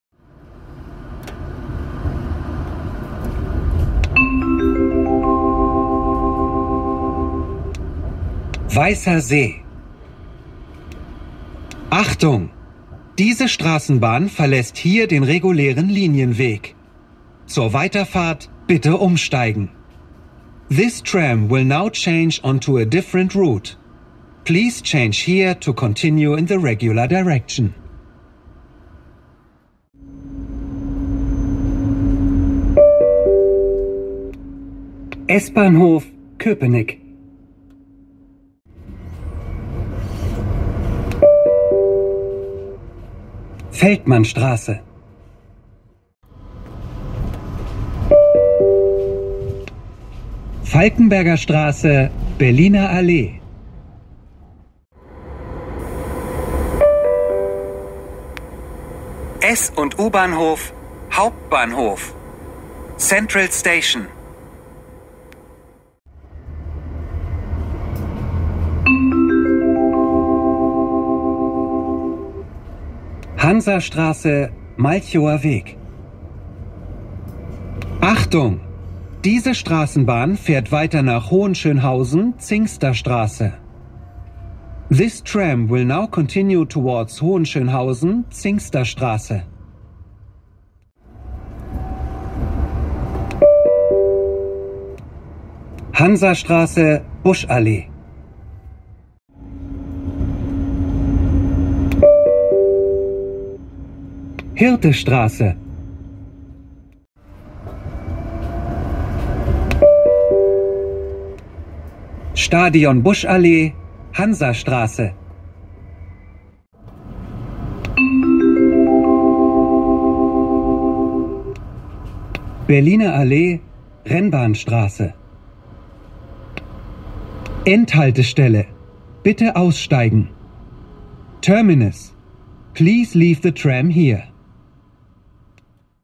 [Straßenbahn Berlin] Neue Ansagen von Philippa Jarke
Hallo! Hier sind die Ansagen die neue Strassenbahn-Ansagen von Philippa Jarke.

Aufgenommen in GT6N und Flexity.

Datum: Juni 2021

Ansagensprecher: Philippa Jarke

Bitte denke daran: Verwenden Sie dieses Videomaterial nicht ohne meine Erlaubnis!